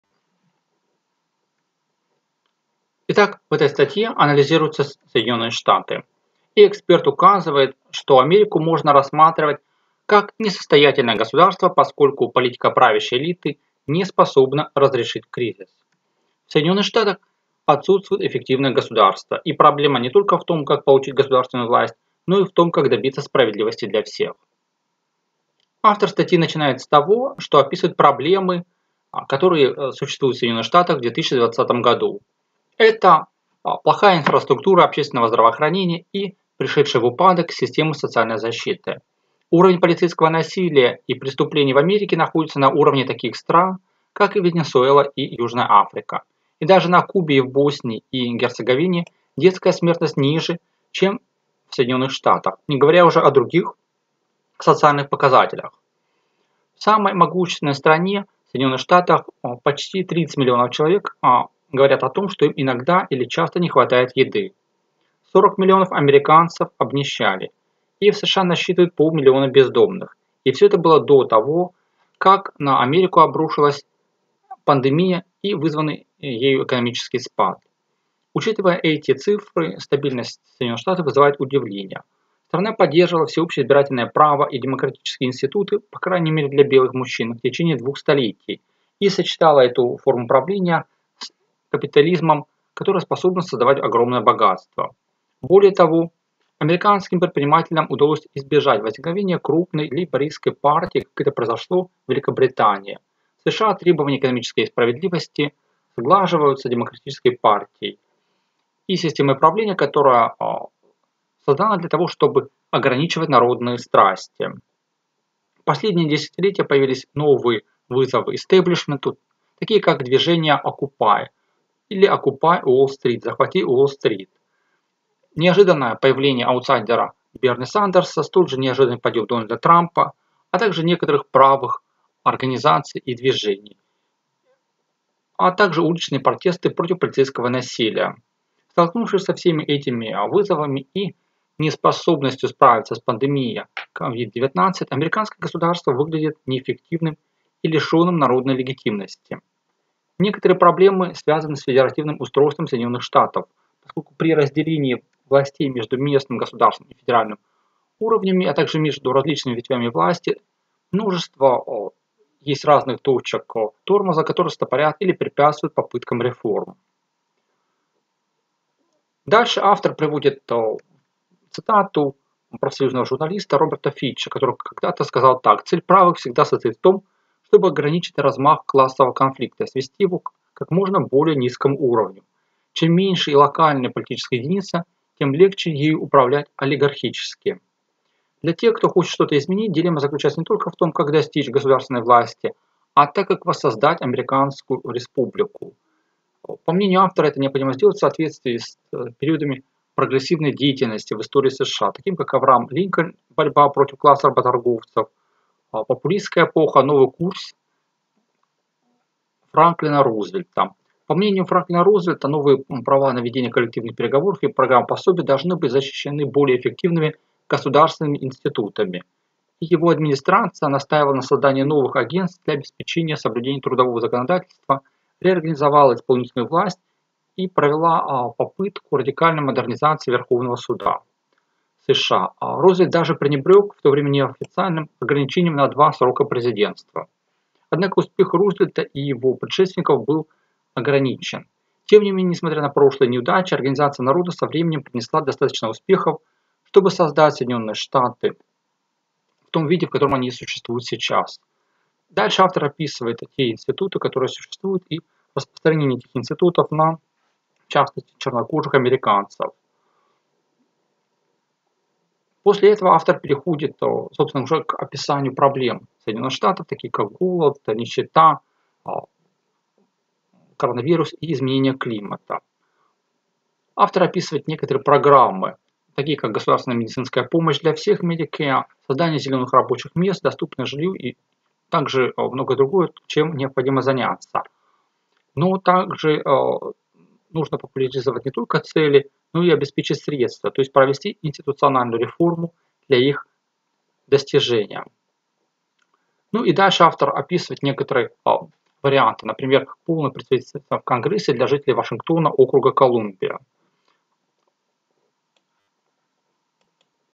США как несостоятельное государство
Экономический и финансовый кризис в США. Социальные и политические процессы, а также влияние пандемии.

Интересные видео